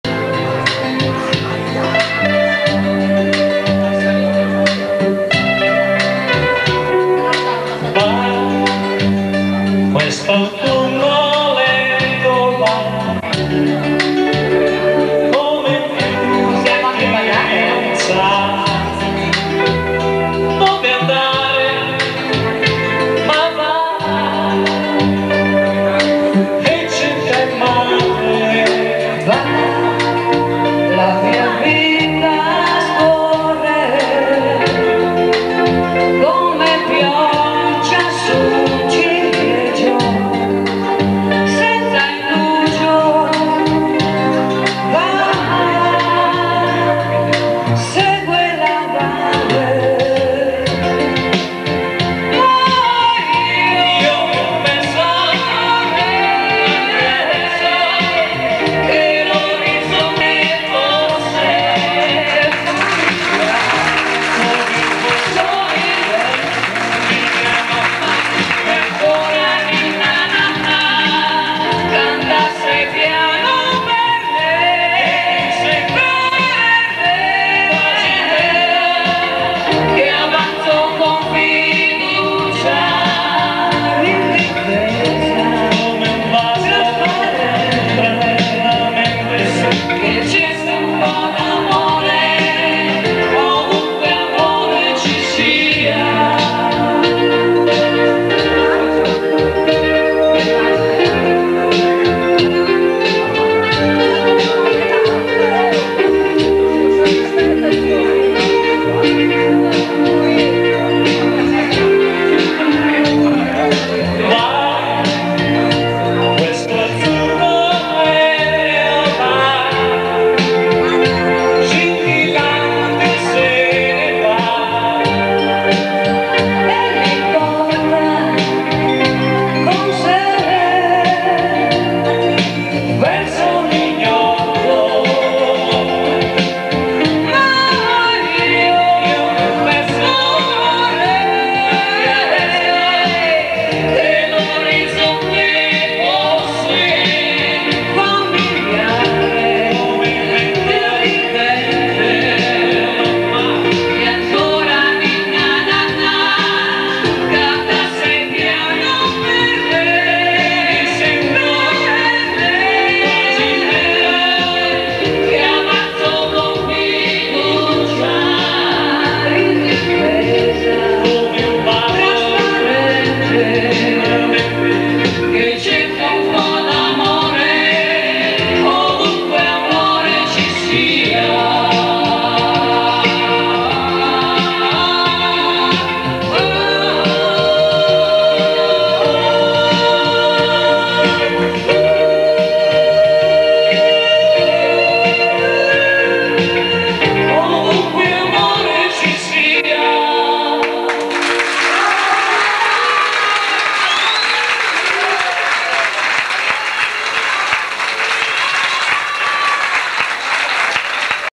Oprandi Adriano e Mary Sonzogni
Oprandi Adriano alle Tastiere e Mary Sonzogni
stanno suonando per uno sposalizio maggio 1990